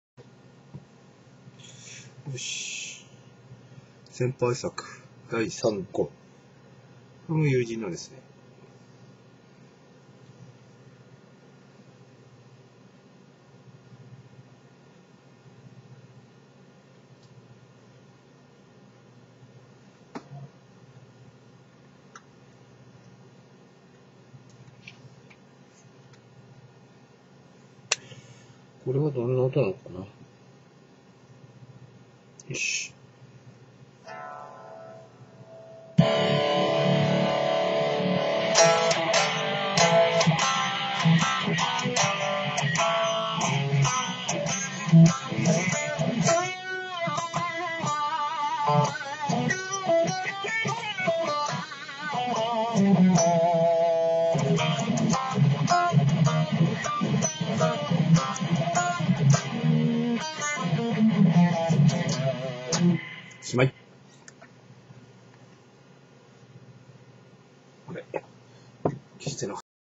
先輩が自作した 3号機 VAN HALENヴァンヘイレンモデル
先輩が自作したVAN HALEN 3号機です。これも友人の物です。Webカメラの音源なんで音は悪いですけど＾＾；JM4を使って録音するといいんですけどねｗ